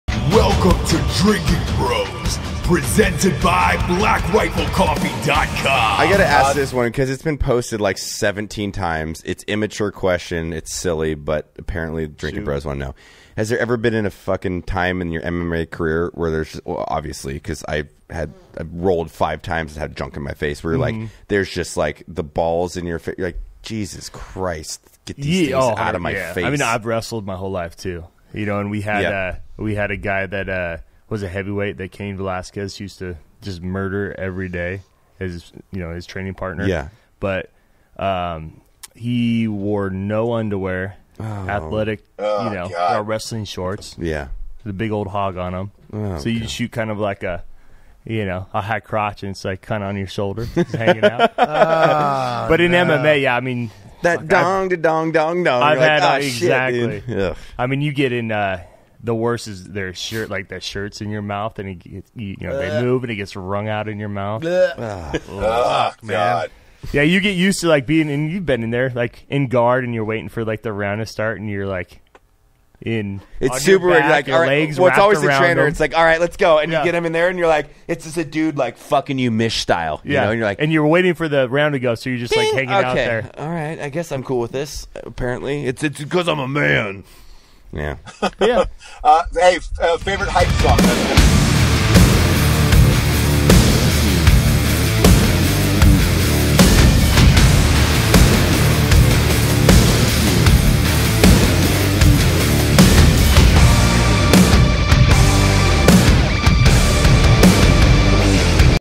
Ryan Bader and Mat Best: Ever fought with a D*ck in your face?
Ryan Bader joins us in the studio as Mat Best asks him questions provided by the Drinkin Bros community.

Drinkin’ Bros Movie:
Drinkin Bros Live: The Shaved Eagle Tour